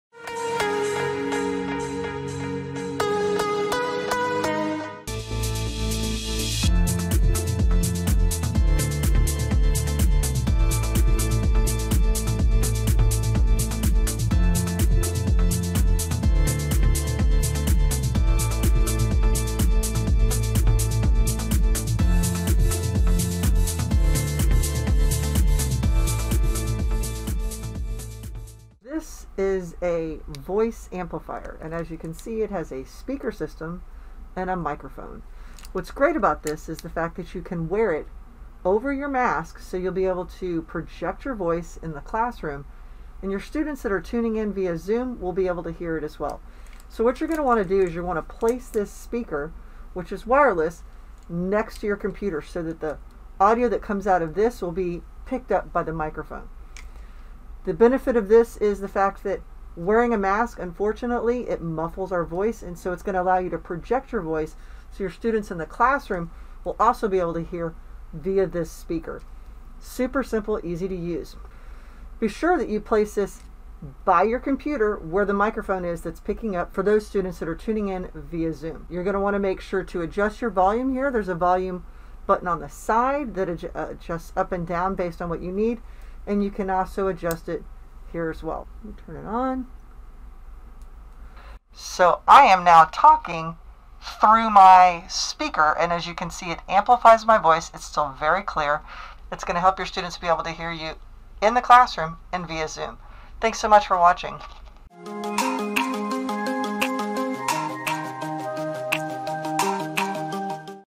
Voice Amplifier
Tech Tips and Tricks for Teachers Video Series:

In today's video, Jen is back to share all about how to use a voice amplifier to amplify your voice in the classroom!  Voice amplifiers make it easy for your students to hear you when wearing a mask while teaching and Jen shows you how easy they are to use.  You can follow Jen on Twitter at @apsitjen.

Follow the Atlanta Public Schools' Department of Instructional Technology on Twitter at @APSInstructTech.  We look forward to connecting with you there!